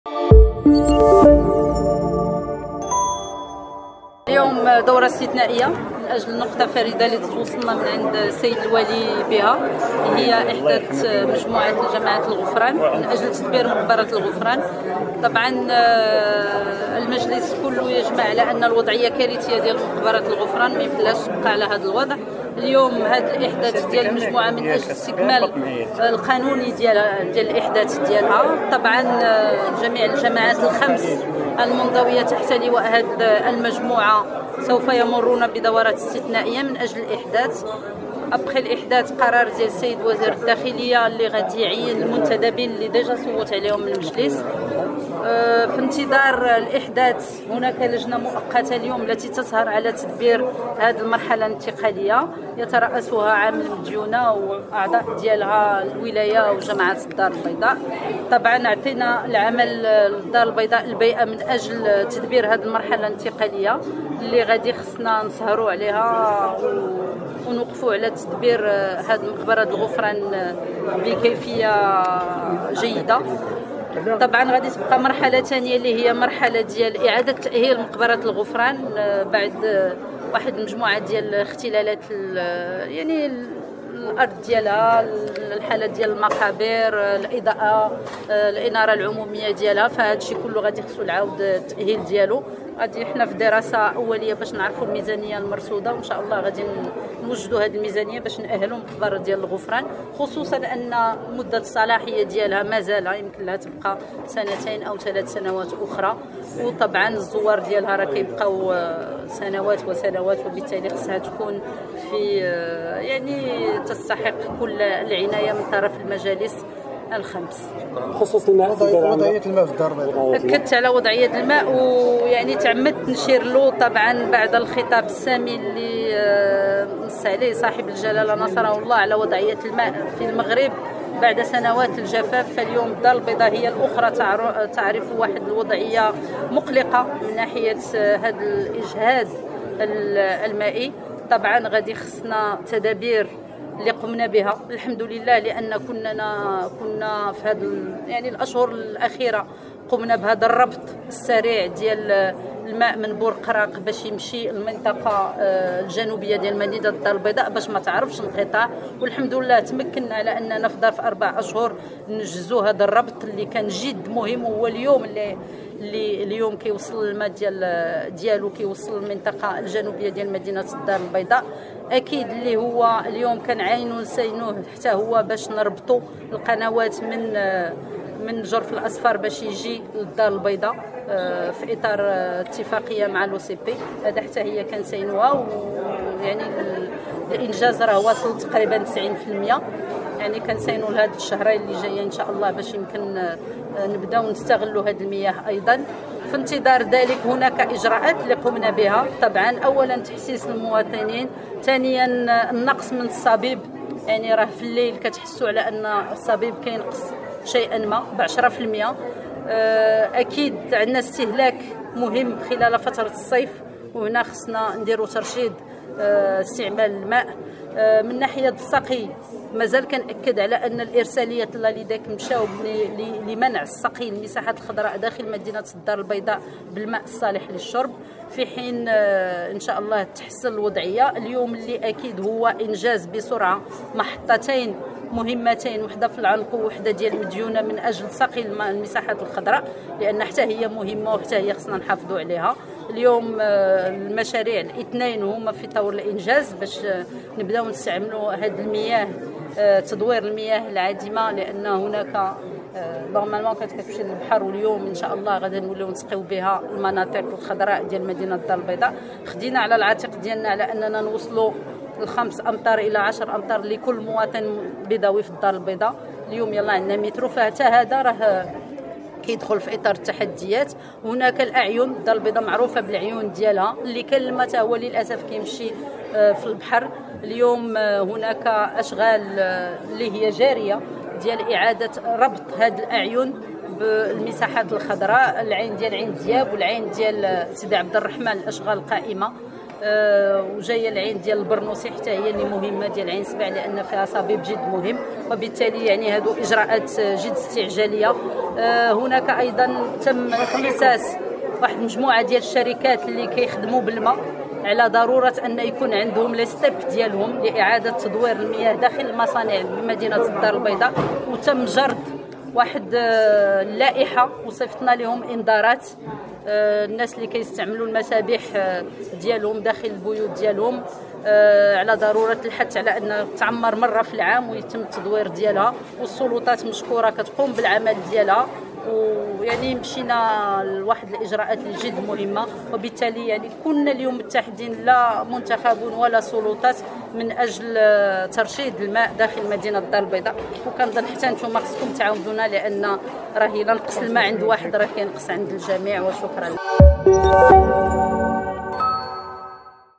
العمدة ارميلي: الوضعية المائية في الدارالبيضاء مقلقة وهذه أهم الإجراءات المتخذة
أكدت نبيلة ارميلي، عمدة مدينة البيضاء، أن الوضعية المائية في العاصمة الاقتصادية مقلقة وأن هناك مجموعة من التدابير التي تم اتخاذها من أجل تقليص من حجم الأزمة المائية في المدينة